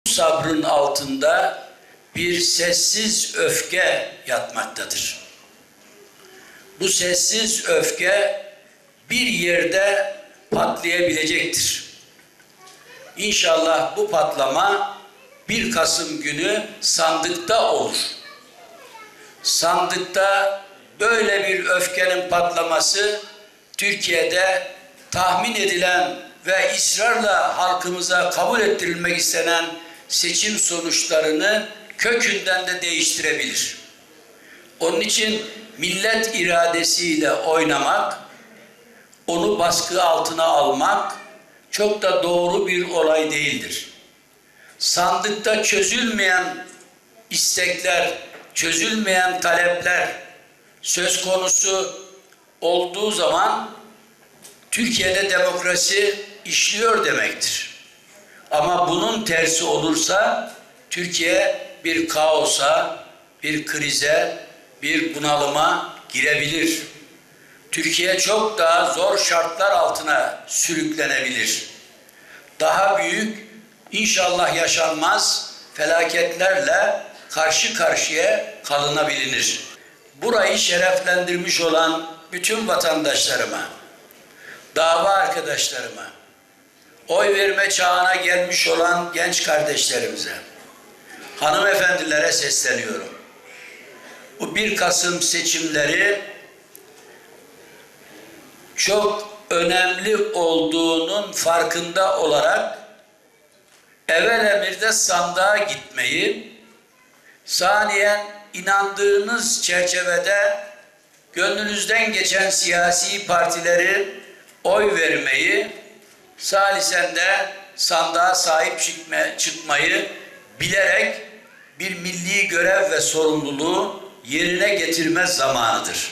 Bahçeli: Patlama İnşallah Sandıkta Olur
MHP Genel Başkanı Devlet Bahçeli, ''İnşallah bu patlama 1 Kasım günü sandıkta olur. Sandıkta böyle bir öfkenin patlaması Türkiye'de tahmin edilen ve ısrarla halkımıza kabul ettirilmek istenen seçim sonuçlarını kökünden değiştirebilir'' dedi.